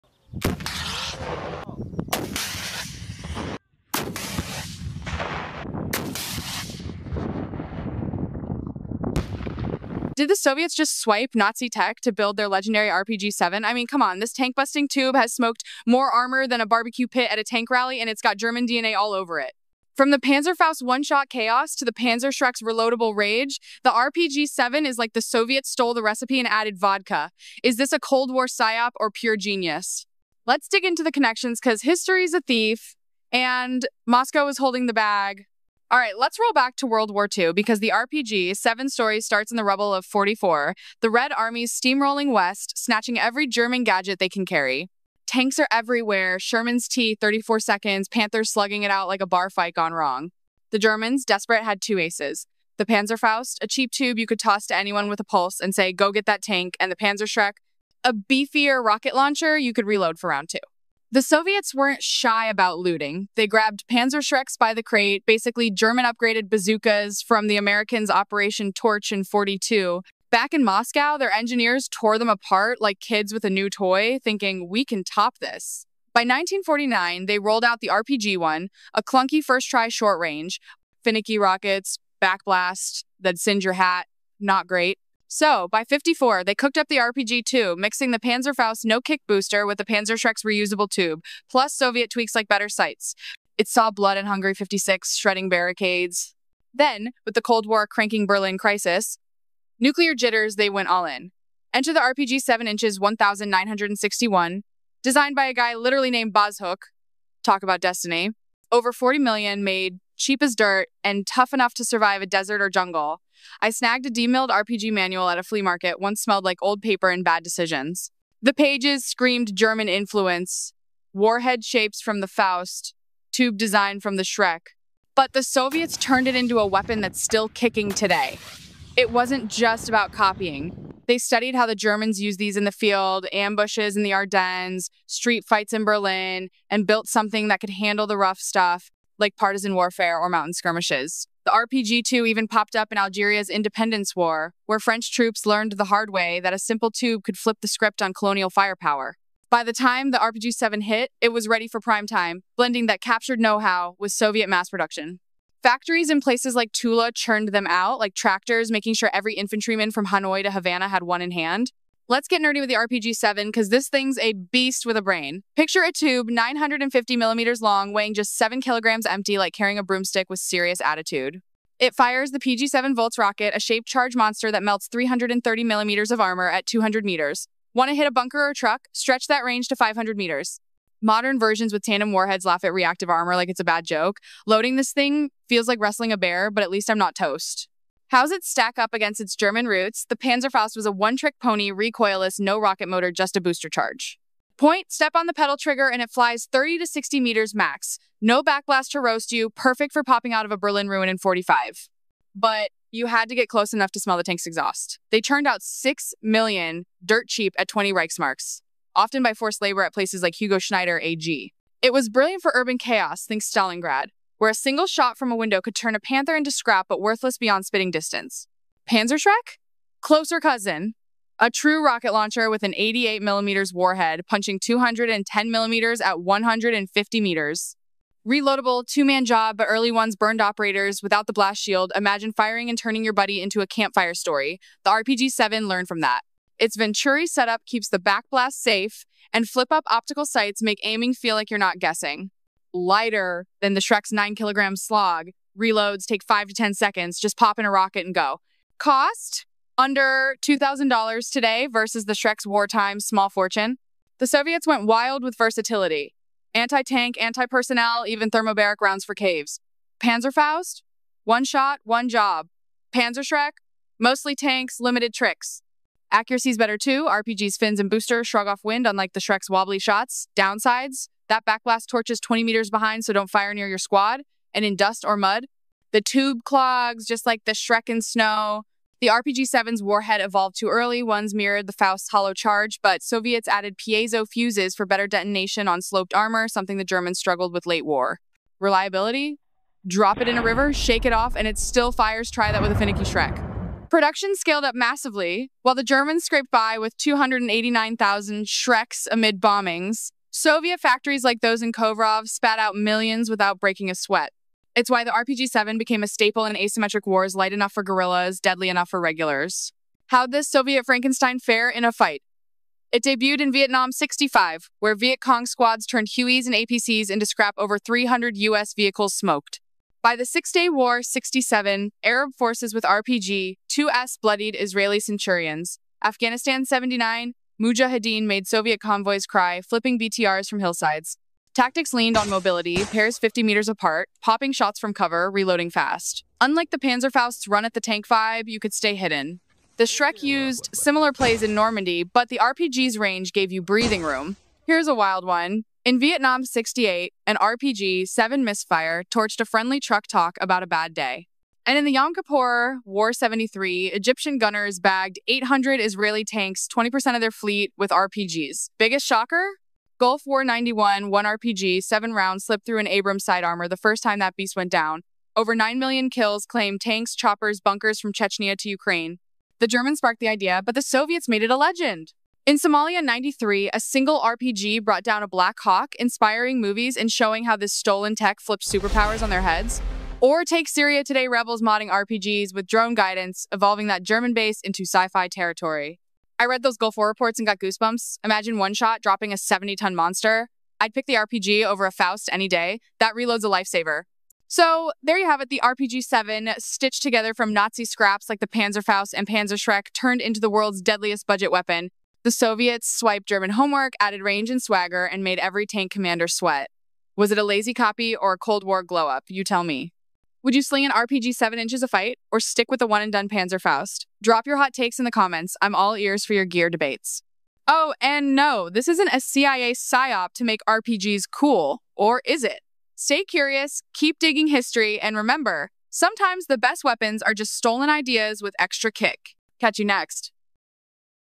Did SOVIETS Steal the RPG-7 from NAZI Germany? Panzerfaust vs Panzerschreck EXPOSED
Did the Soviet Union steal Nazi Germany's anti-tank weapon technology to create the legendary RPG-7?

This video exposes the shocking truth about how Soviet engineers reverse-engineered captured German Panzerfaust and Panzerschreck weapons to build the world's most prolific tank killer—with over 40 million units produced and still dominating battlefields today.
🔥 FROM NAZI GERMANY TO SOVIET SUPREMACY
When the Red Army captured Berlin in 1945, Soviet engineers seized thousands of German anti-tank weapons. The Panzerfaust (6 million produced, 20 Reichsmarks each) and Panzerschreck (289,000 units with 88mm warheads) became blueprints for the RPG-7—the most successful anti-tank weapon in history.
⚔️ THE EVOLUTION OF THE RPG-7
German Predecessors:

Panzerfaust: Disposable, 30-60m range, recoilless design, perfect for urban warfare
Panzerschreck: Reloadable, 150m range, 210mm armor penetration, reverse-engineered from American Bazookas

Soviet Development:

RPG-1 (1949): First clunky attempt with dangerous backblast
RPG-2 (1954): Combined best features, saw combat in Hungary 1956 and Algeria
RPG-7 (1961): Perfected design—7kg weight, 330mm penetration, 200-500m range, under $2,000 cost

💥 LEGENDARY COMBAT RECORD
Vietnam War: Destroyed 300+ U.S. vehicles including Hueys and APCs
Yom Kippur War (1973): Egyptian forces destroyed 800 Israeli tanks—20% of their entire fleet
Soviet-Afghan War: Mujahideen devastated Soviet convoys from mountain positions
Gulf War (1991): First confirmed M1 Abrams tank kill
Modern Conflicts: Over 9 million confirmed kills across Ukraine, Syria, Iraq, Afghanistan
🎯 WHAT MAKES IT SUPERIOR?
The RPG-7 surpasses its German predecessors through:

Reloadability: 5-10 second reload vs Panzerfaust's single shot
Extended Range: 500m vs 60m maximum
Versatile Ammunition: Anti-tank, anti-personnel, thermobaric rounds
Modern Upgrades: Tandem warheads defeat reactive armor
Extreme Reliability: Functions in desert, jungle, arctic conditions
Mass Production: Soviet factories in Tula and Kovrov produced millions efficiently

📚 KEY QUESTIONS ANSWERED:

How did Soviet engineers access and study captured Nazi weapons?
What specific German features were copied versus innovated?
Why does the RPG-7 remain effective against modern 2025 tanks?
How did Soviet mass production capabilities change warfare?
What tactical innovations separated the RPG-7 from German designs?
Were the Soviets thieves or brilliant engineers?

🔍 TECHNICAL DEEP DIVE INCLUDES:
Detailed warhead comparisons, shaped-charge technology evolution, Soviet reverse-engineering process, German field tactics that influenced Soviet designs, and how the RPG-7 evolved from simple hollow charges to advanced piezo fuses. We examine factory production at Tula and Kovrov, combat effectiveness from World War 2 to modern conflicts, and modifications including drone guidance systems used in Syria.
Was this Cold War espionage or engineering genius? From Berlin's rubble in 1945 to Ukrainian trenches in 2025, discover how Soviet engineers transformed German homework into a weapon that changed warfare forever—and why it's STILL the world's most feared anti-tank weapon.